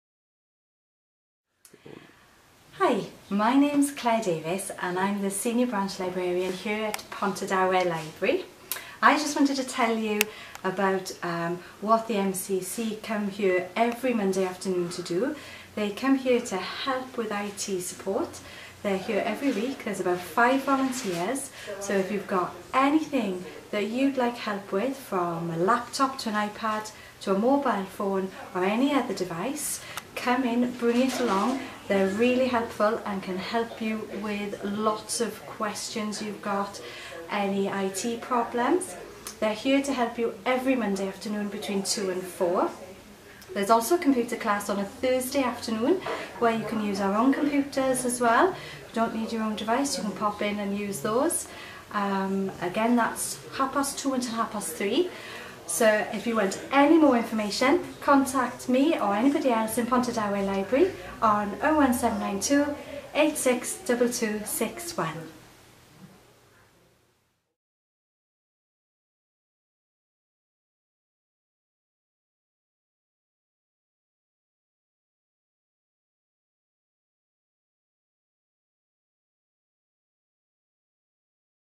Pontardawe Library
Short film about Get NPT Online sessions taking place in Pontardawe Library. Directed by Richard Jack Smith